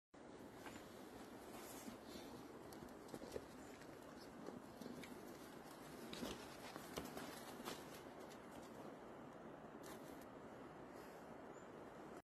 What it's like to be a kiwi ranger in Fiordland, NZ
This is leatherwood, and navigating it to look for kiwi is no easy task. Just ask Monty.

Monty and Doddy (who's in the background) are in the Fiordland Kiwi Diaries miniseries, out now!